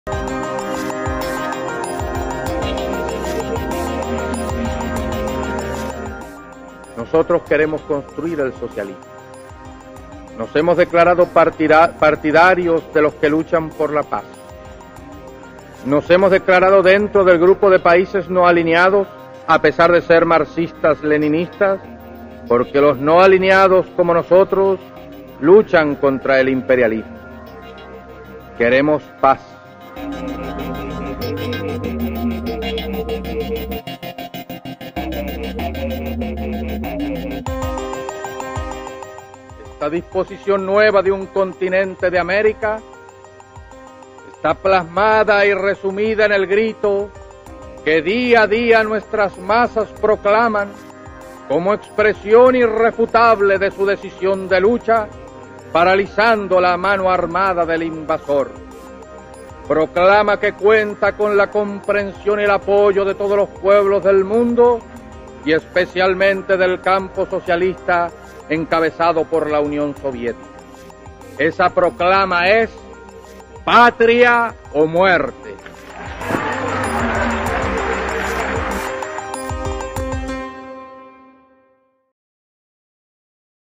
Che Guevara: "Patria o Muerte"
Snapshot | An iconic moment in the United Nations happened when Che Guevara took the stage.